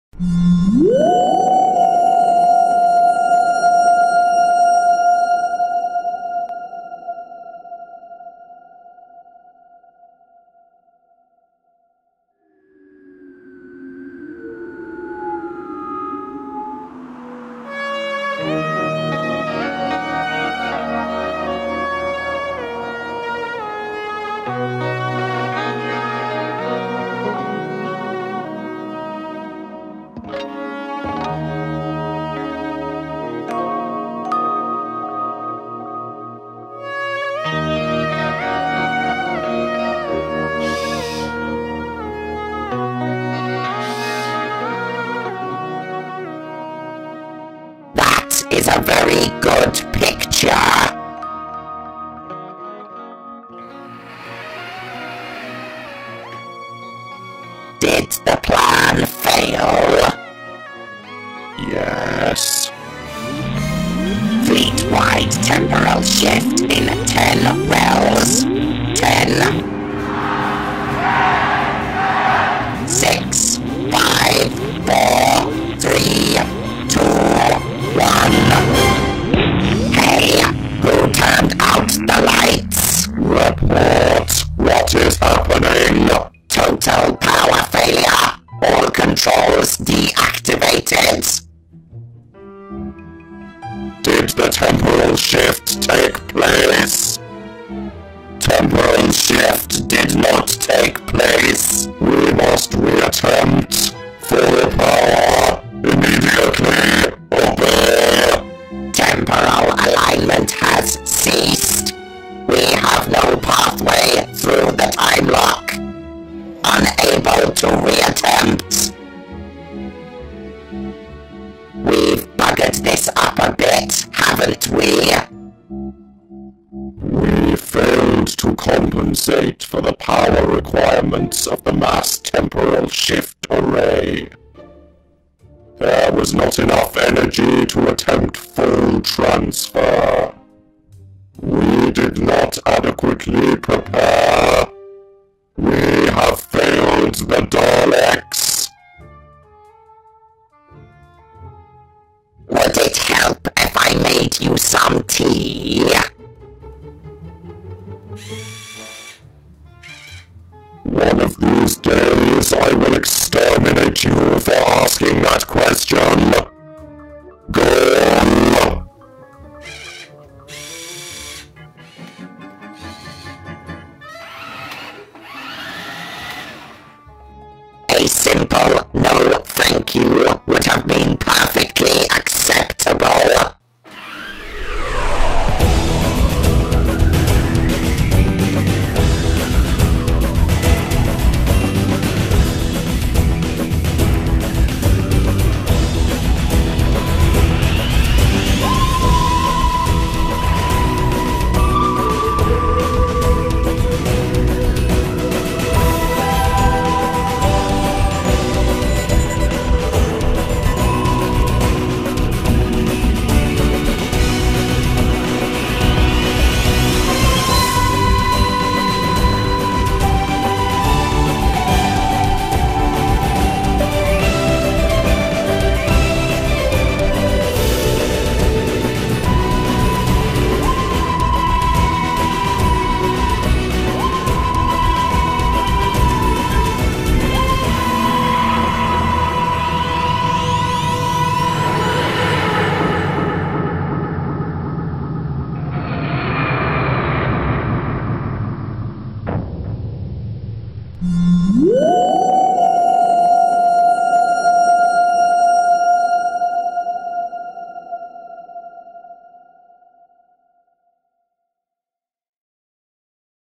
New Years Day
1st January 2014, 11:55am. Did the Daleks succeed?

This video is part of Dalek December, celebrating 50 years of Doctor Who and the Daleks.